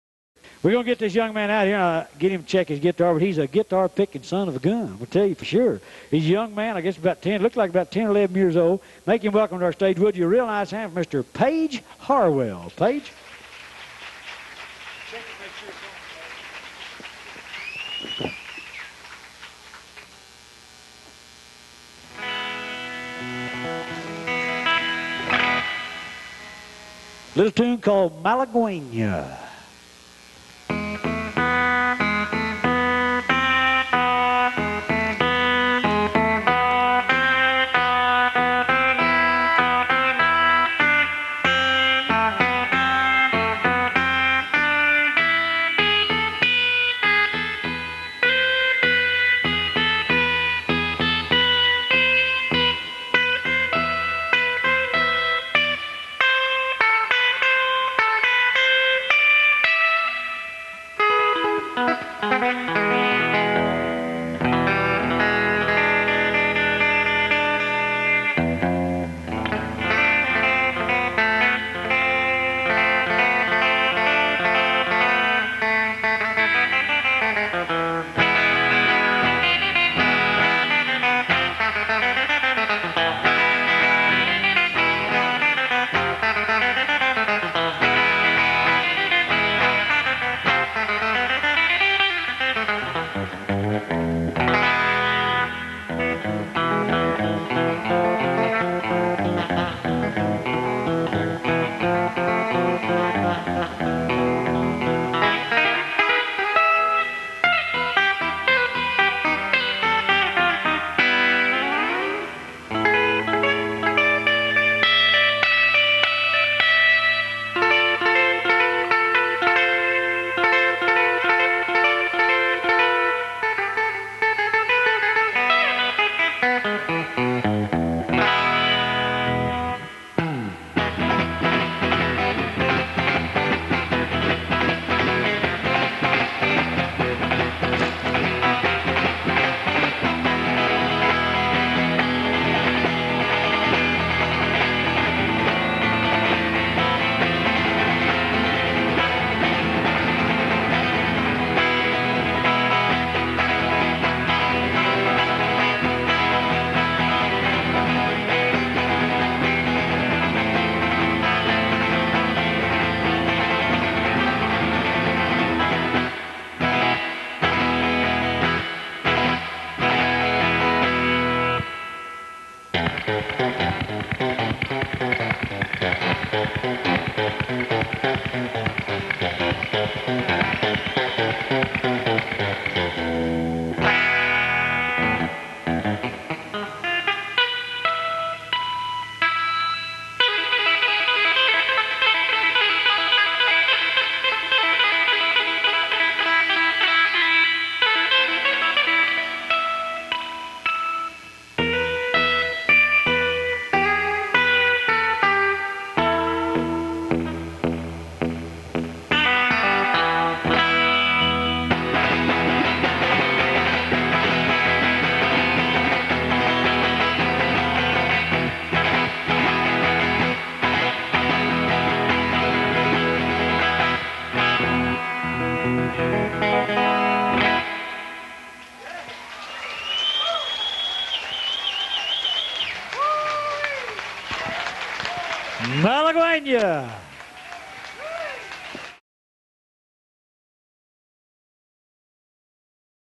Paige Harwell 1980-Stephenville Opry
Paige performing at age 11 at the Stephenville Opry in TX...Paige's dad and sister performed on this show as well...